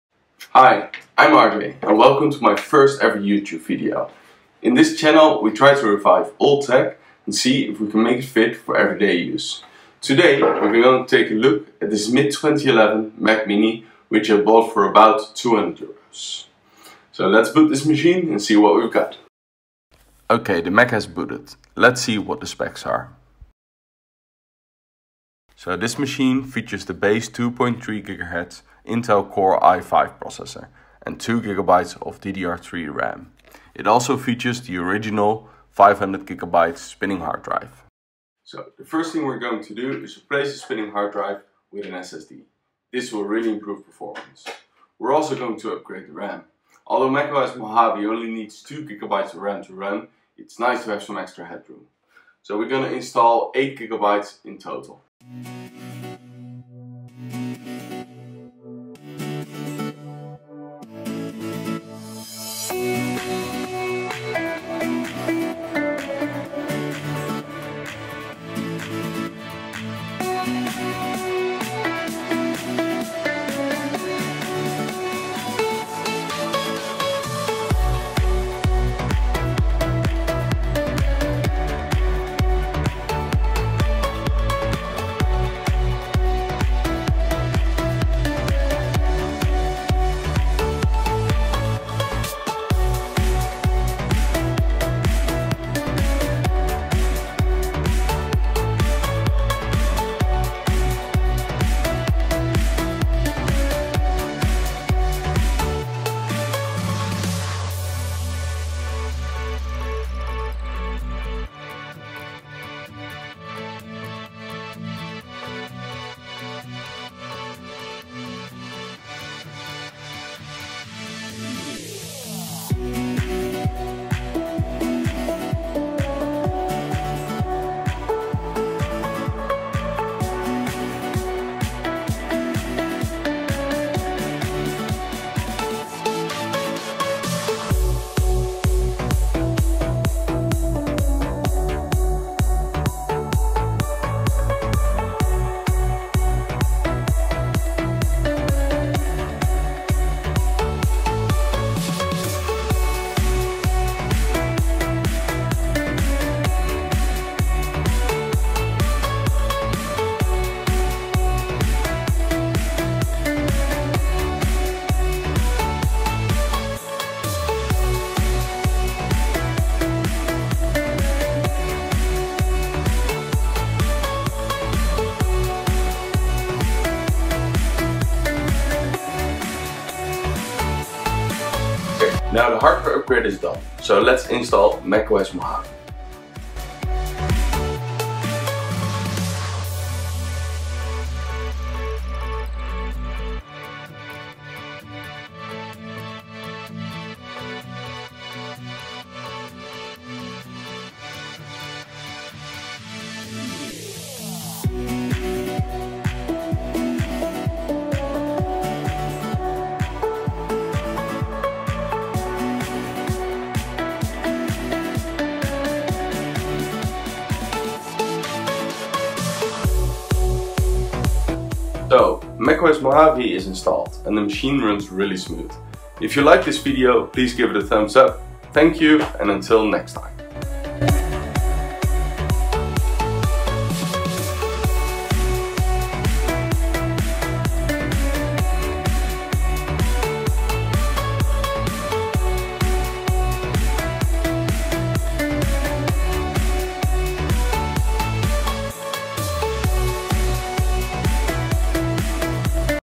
Upgrading a 2011 Mac Mini for use in 2019!
In this video we're going to upgrade a mid 2011 Mac Mini for use in 2019.